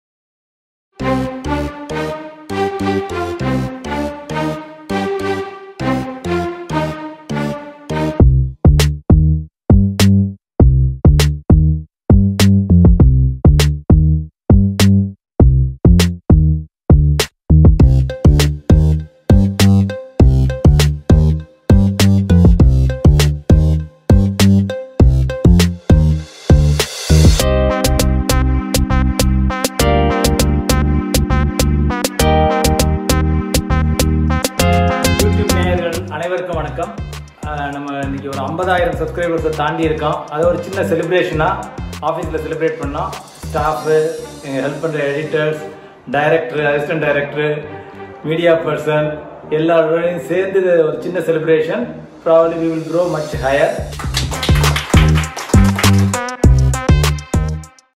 Prakala Wealth 50,000 Subscribers Celebration - A Big Thank You! Look Forward 4 Your Support
We are very thankful for your support and patronage. Without you, we could not have been there. Let us know the topics of your interest and will upload accordingly.

Yours,
Chokkalingam Palaniappan